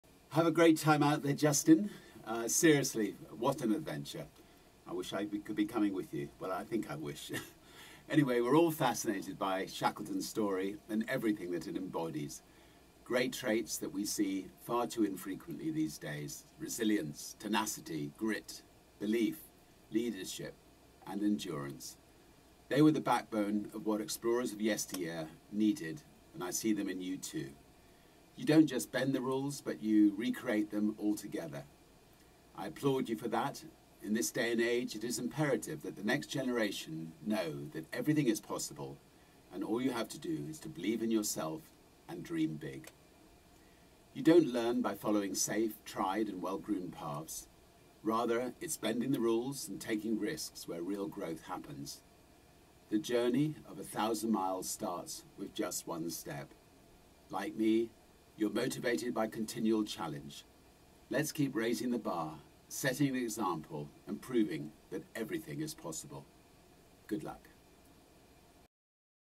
BendTheRules Richard Branson’s support for Justin Packshaw
You don't learn from following safe tried and well groomed paths. Rather its bending the rules and taking grit where real growth happens. The journey of a thousand miles starts with just one step. Richard Branson sends his support to Justin Packshaw as he embarks on an epic adventure of endurance to Antarctica. Join in an show your support for Justin with Bendtherules